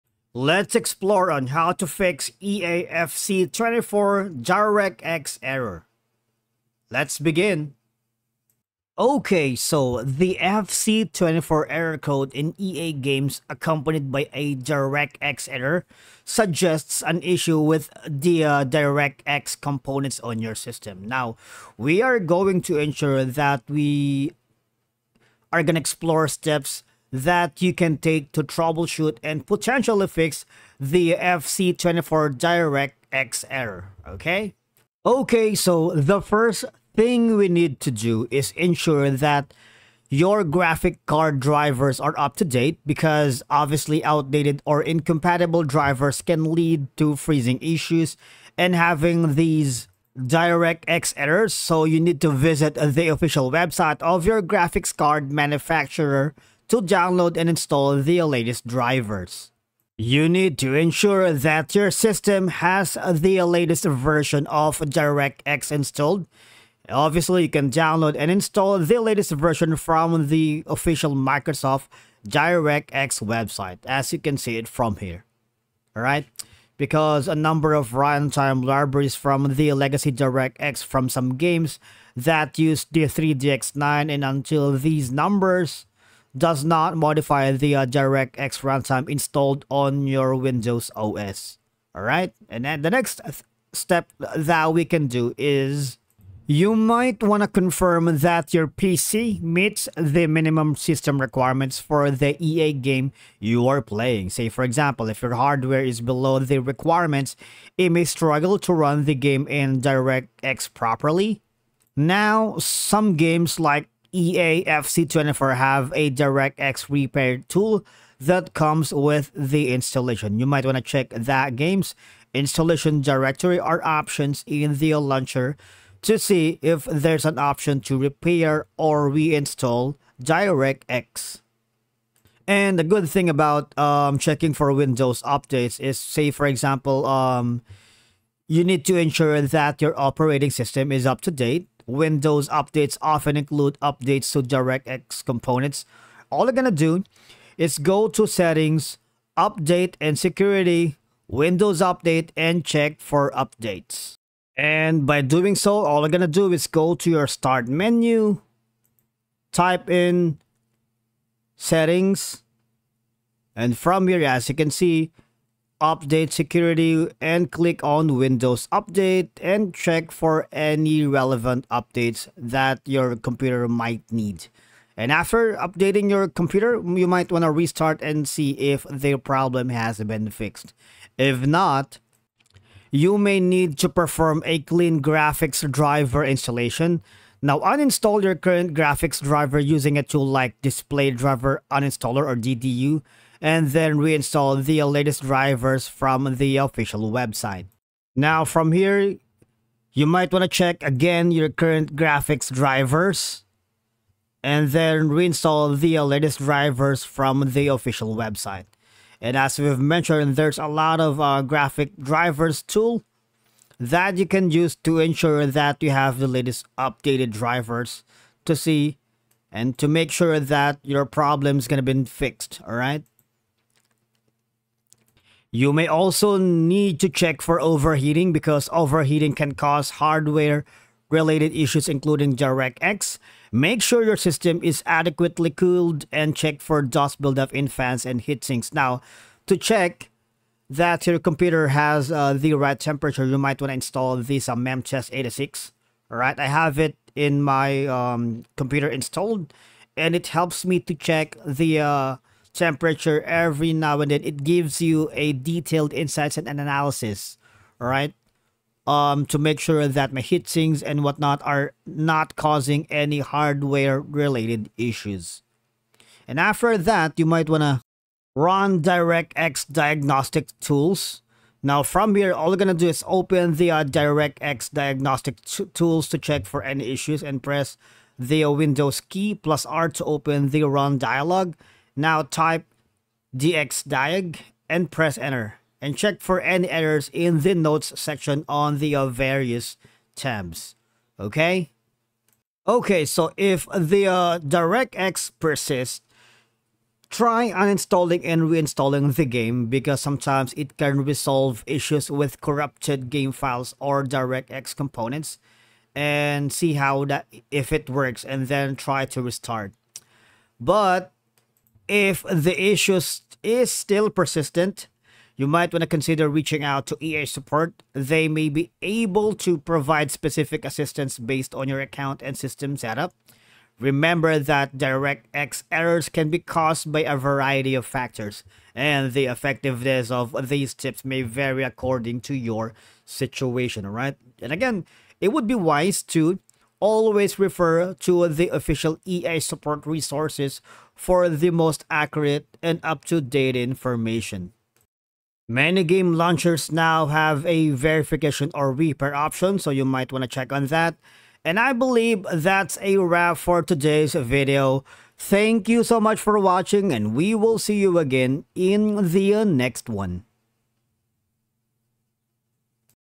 How to Fix FC 24 Directx Error 2025 Update - Full Guide
Encountering the dreaded FC 24 DirectX error? Fret not! In this video, we provide the latest 2025 update and a step-by-step guide on how to fix the FC 24 DirectX error, ensuring you get back into the game smoothly. 🕹️✨